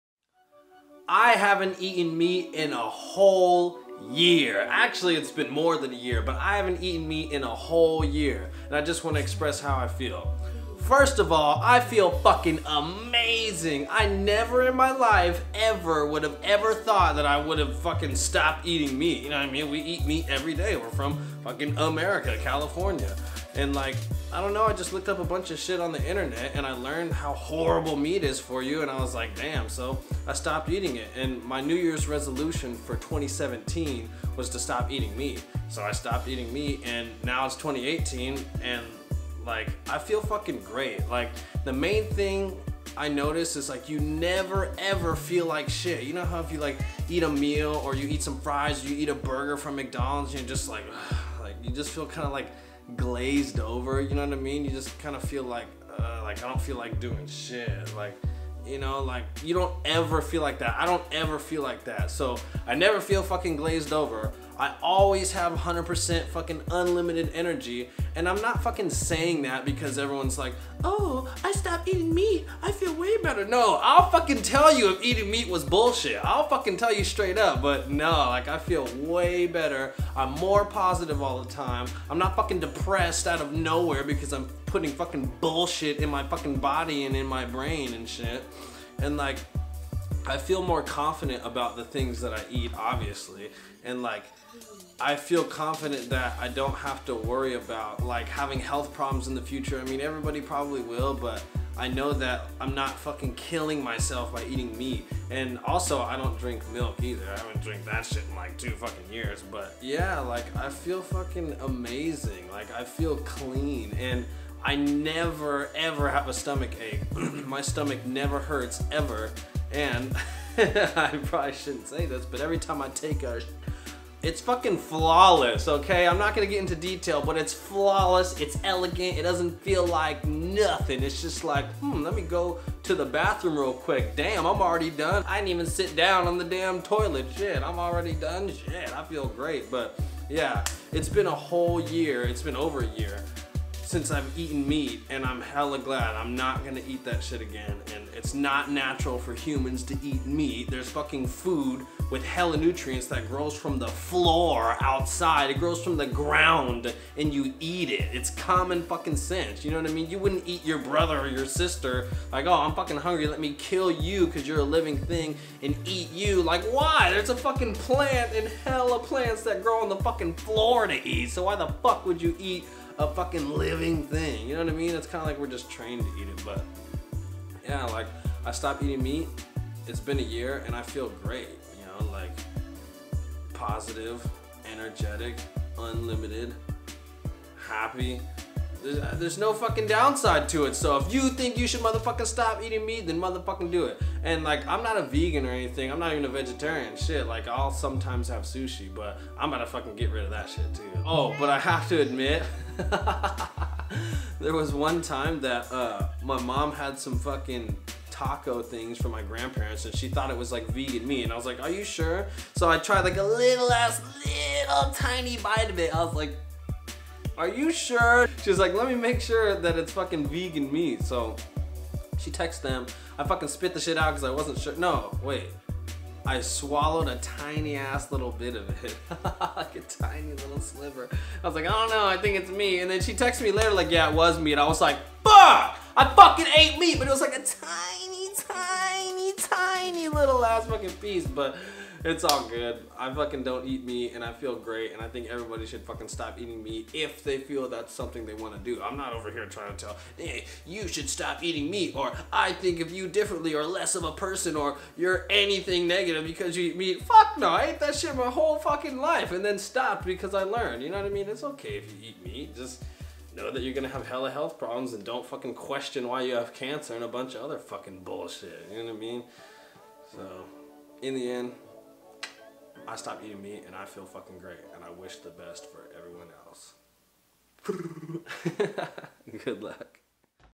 NO MEAT FOR A YEAR!!! How Do I Feel?
I haven't eaten meat in over a year and this is how I feel.